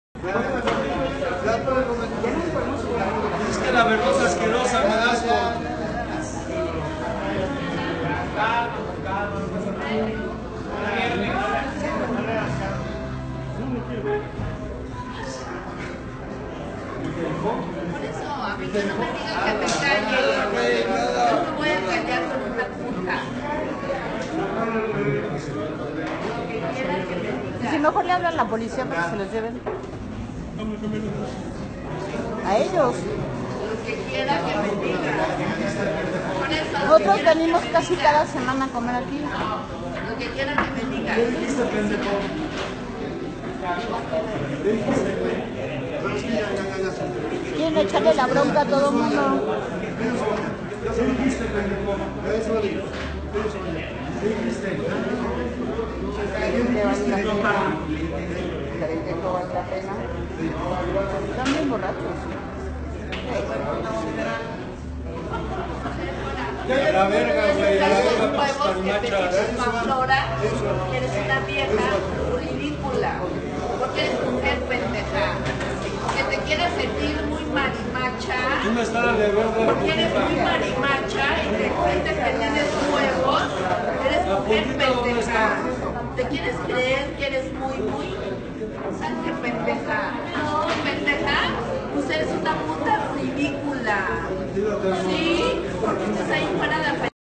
LADY MARIMACHA Y DON VERGAS
UNA MUESTRA MÁS DE QUE EN PLENO SIGLO XXI LA GENTE SIGUE SIENDO HOMOFÓBICA Y SE SIENTEN CON EL PODER DE AGREDIR FÍSICA Y EMOCIONALMENTE A LAS PERSONAS DE LA COMUNIDAD LGBTTT Y HACEN EL RIDÍCULO INTENTÁNDOLO.
GRACIAS AL PERSONAL DEL LUGAR Y A LA GENTE QUE DEFENDIO A LA CHICA.
DENLE LIKE AL VIDEO SI USTEDES IGUAL DEFENDERÍAN A ALGUIEN SIN IMPORTAR COLOR, GENERO, ETC.
Y NO OLVIDEN COMPARTIRLO PARA QUE MÁS GENTE HAGA CONCIENCIA Y NO HAYA TANTA DISCRIMINACION..
Créditos: Gabriela Bravo Herrera.